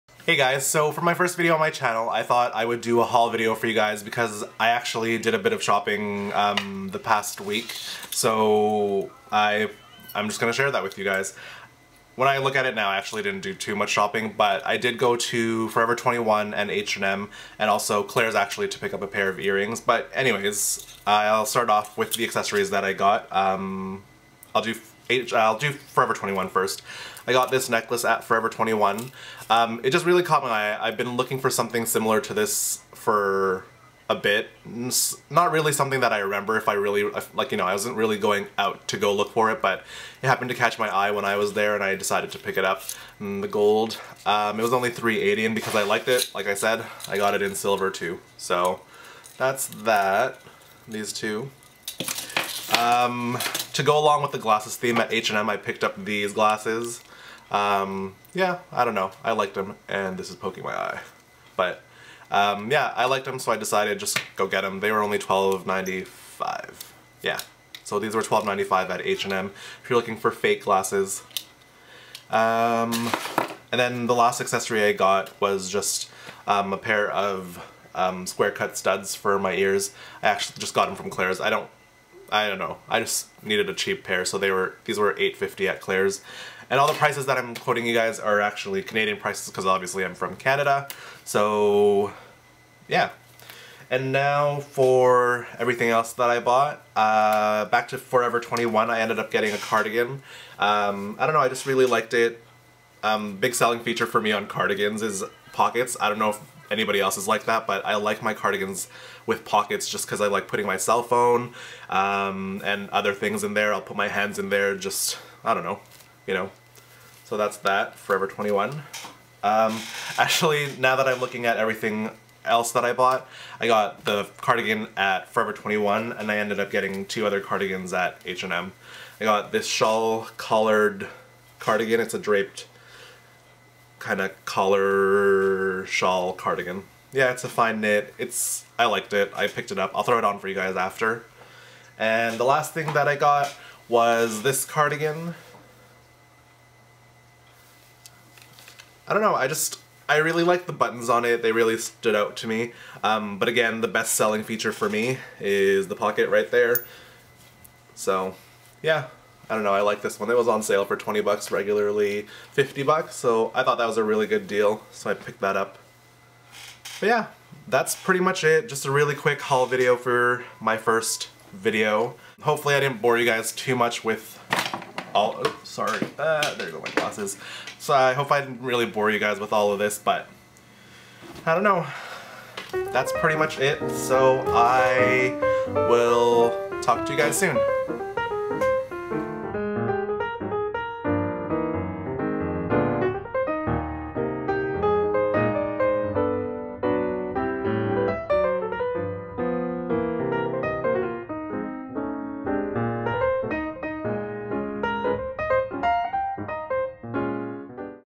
First HAUL Video: Forever 21, H&M & Claire's
My very first haul video!

Forever 21 (21 men)
H&M
Claire's

Thanks for watcthing!